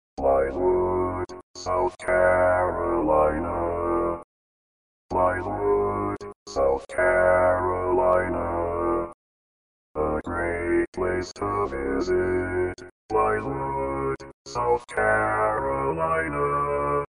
How to Say or Pronounce USA Cities — Blythewood, South Carolina
This video shows you how to say or pronounce Blythewood, South Carolina.

A computer said Blythewood, South Carolina. How would you say Blythewood, South Carolina?